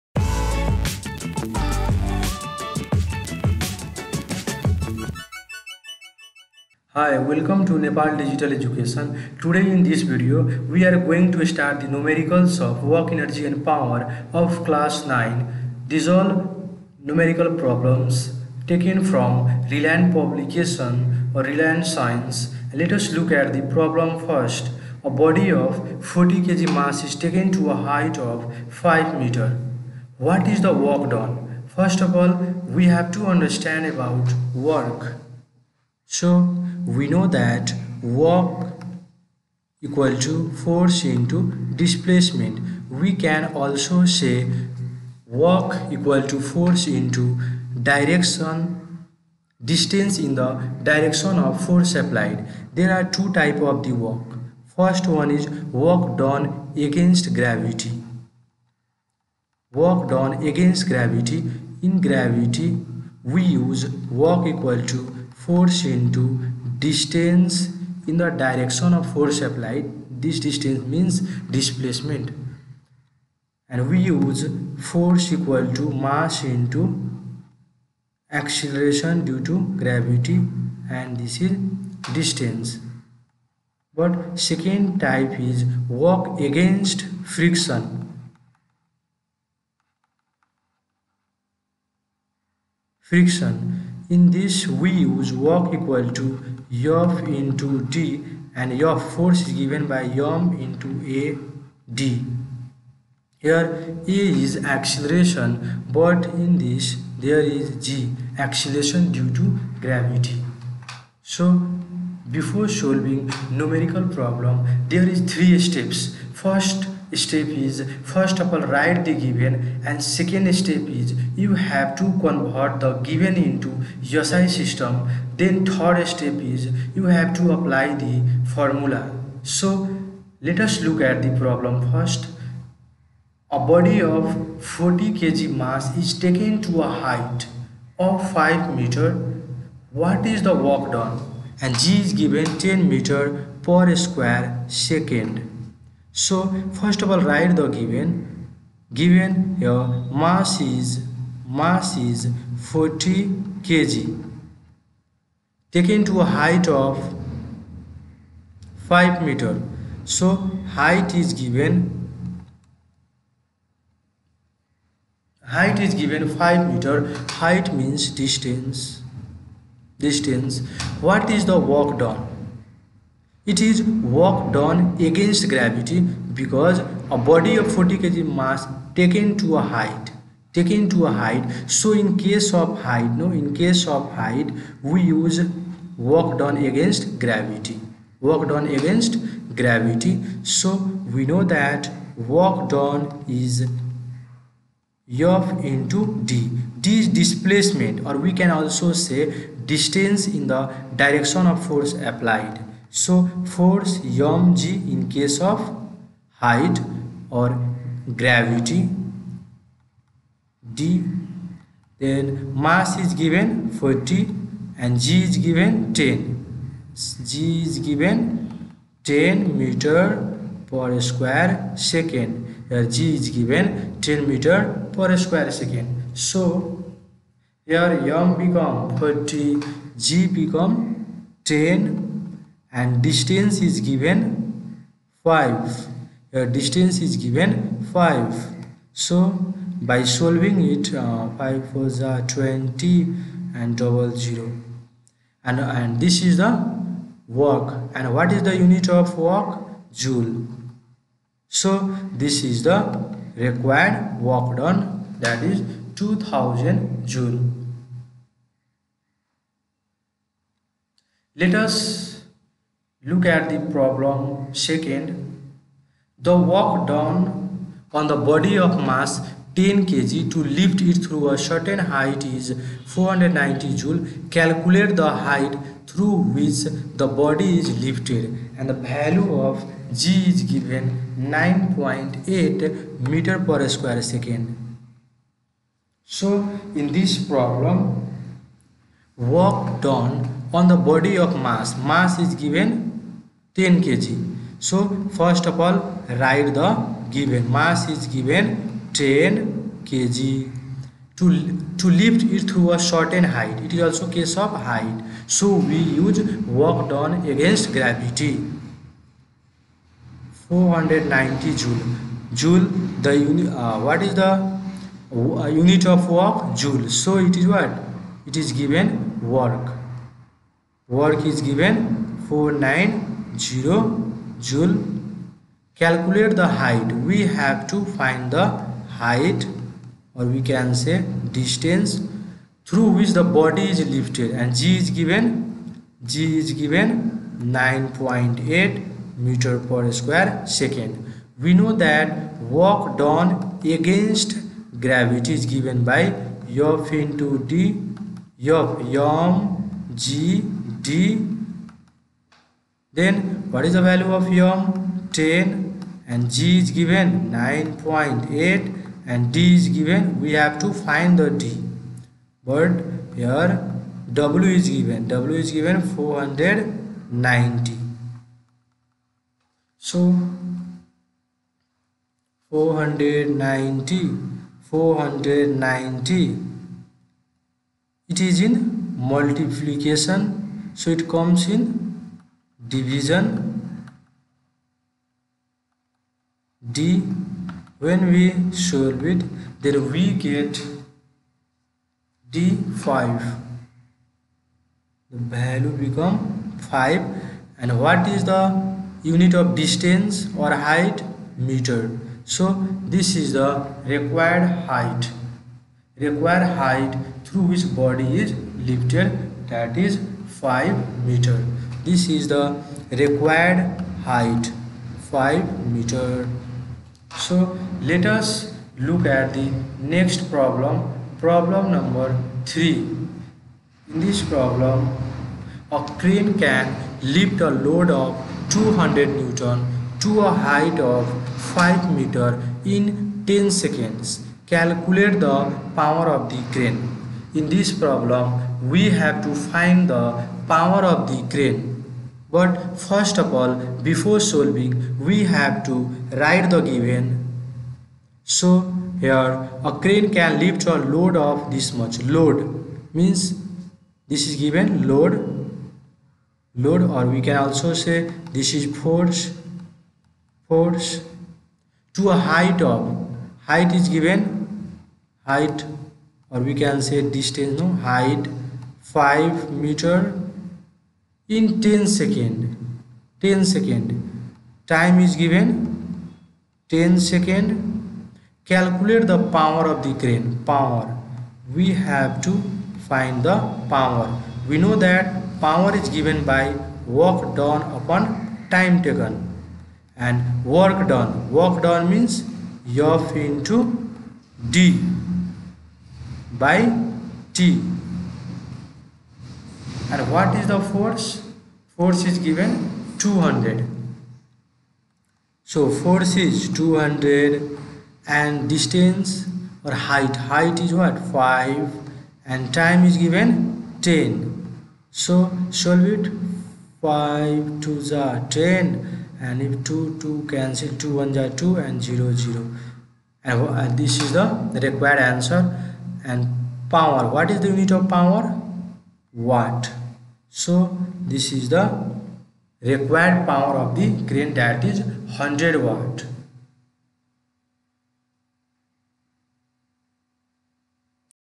PHYSICS(CLASS 9) / WORK, ENERGY AND POWER/NUMERICALS/Problem 1/2/3
RELIANT SCIENCE CLASS 9
LEARN HOW TO SOLVE THE NUMERICALS OF WORK, ENERGY, AND POWER.